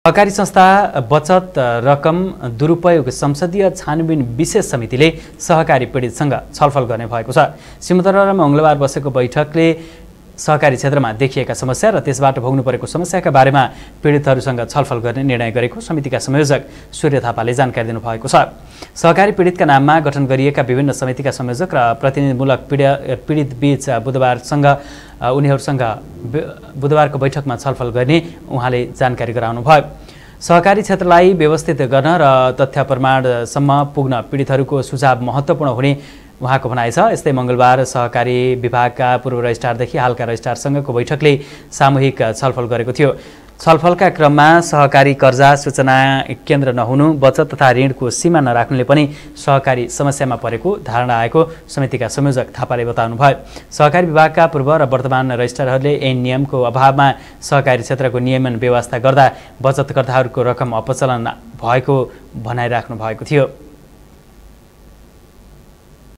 सहकारी संस्था बचत रकम दुरुपयोग संसदीय छानबिन विशेष समितिले सहकारी पीडितसँग छलफल गर्ने भएको छ ।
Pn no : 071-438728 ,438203
Uploaded By : Team M.C.R